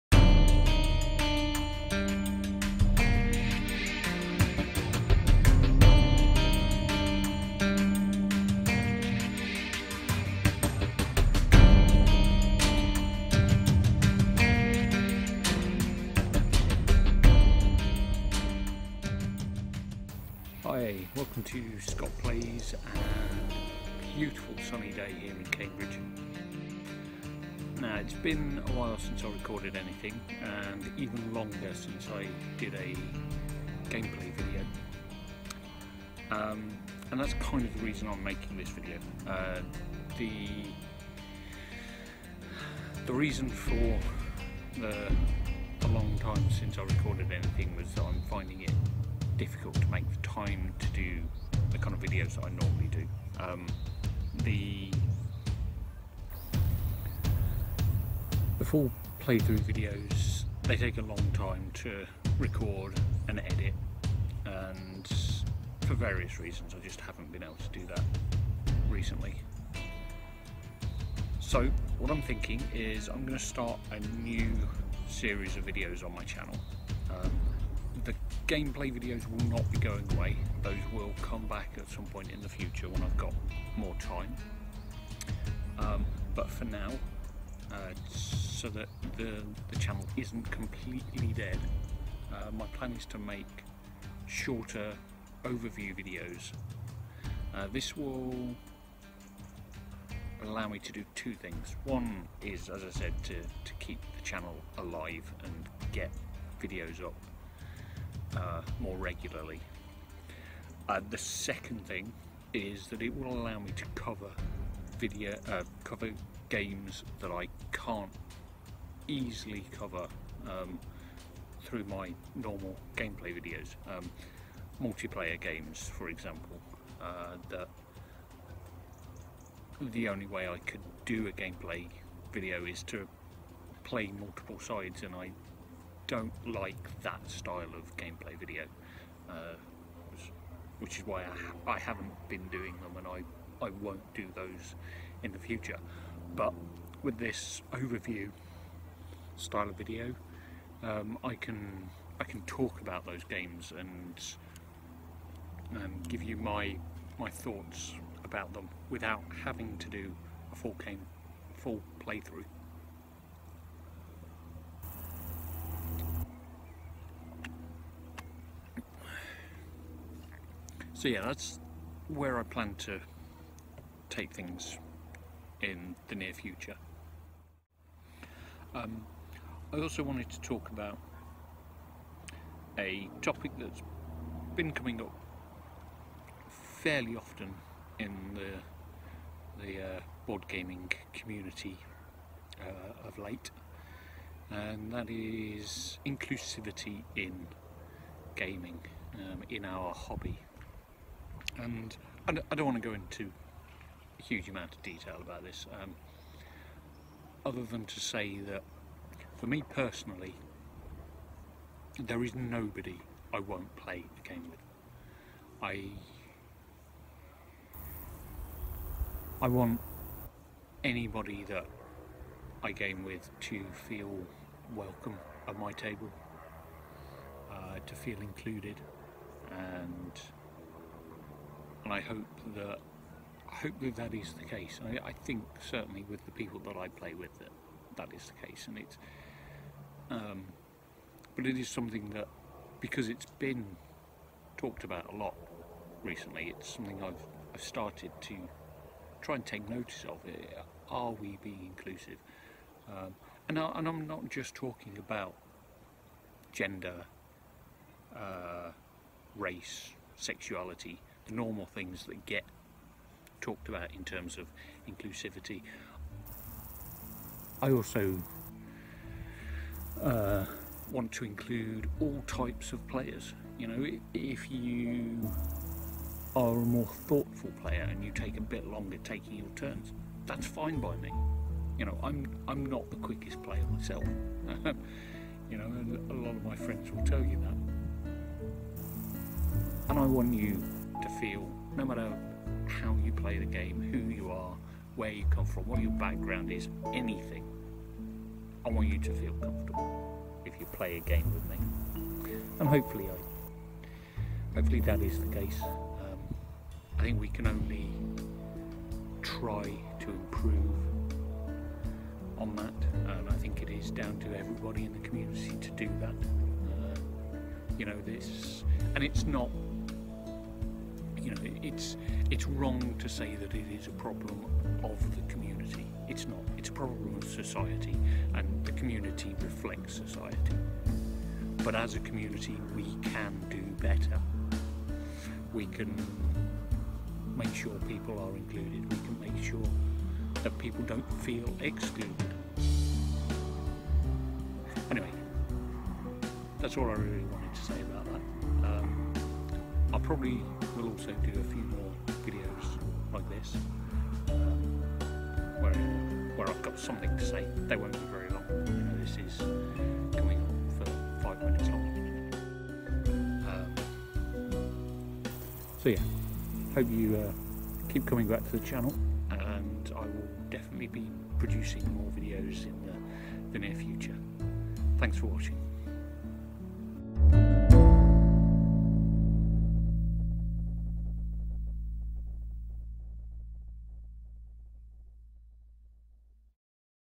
SP10 - July 2018 - New Video Series and Inclusivity in the Hobby
In this video I announce a new video series I have planned for the channel, and discuss my approach to inclusivity in the hobby.